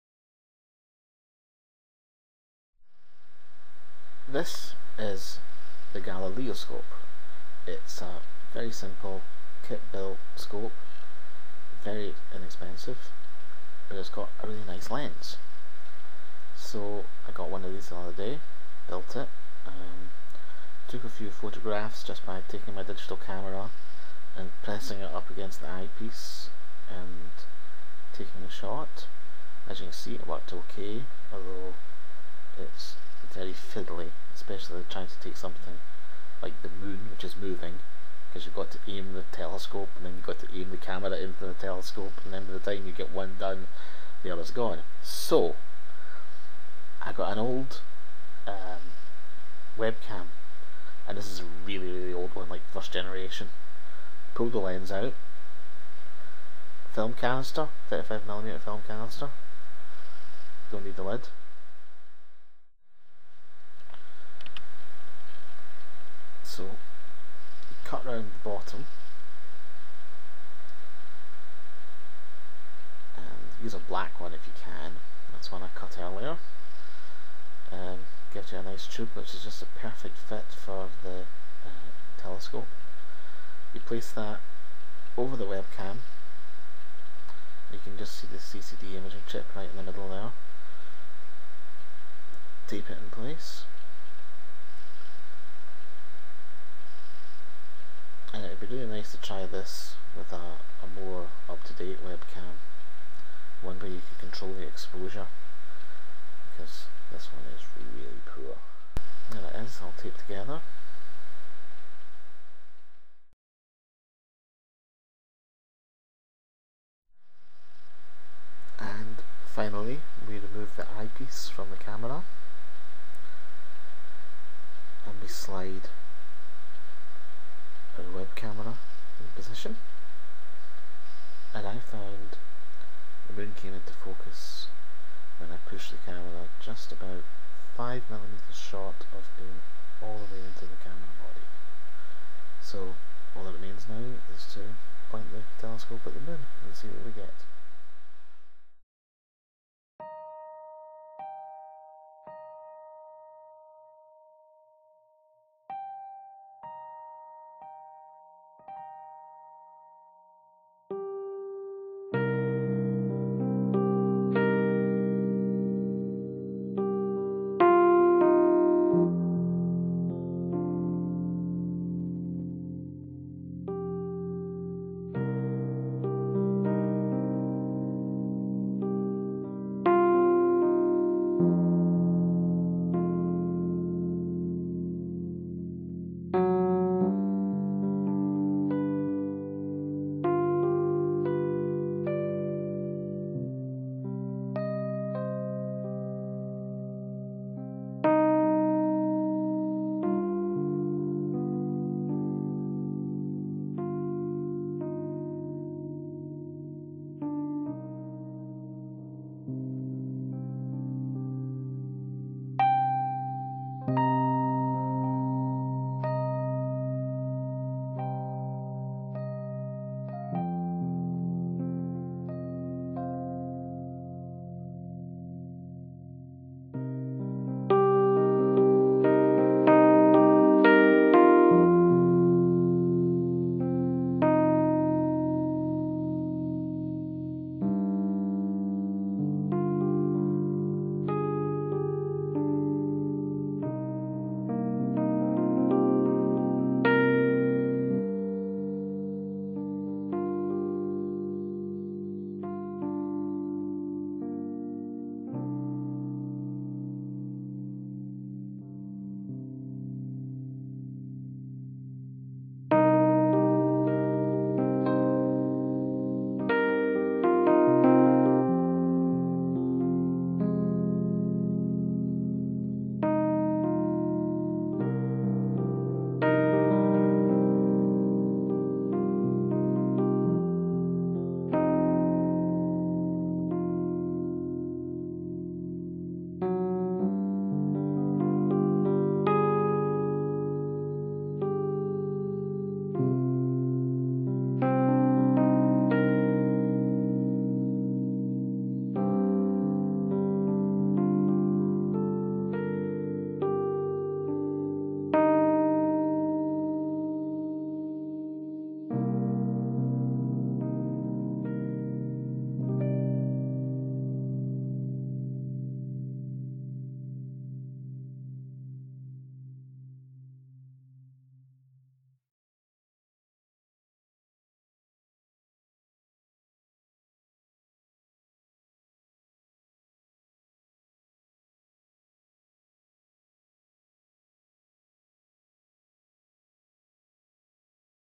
Galileoscope telescope, a web-cam and the moon
I got a Galileoscope the other day and though it'd be fun to attach an old web cam from my bin of obsolete computer junk to it.

If anyone could recommend a good inexpensive web cam that allows manual control of the exposure/brightness/contrast and has a decent resolution I'd love to hear about it. because this one is baaaaad.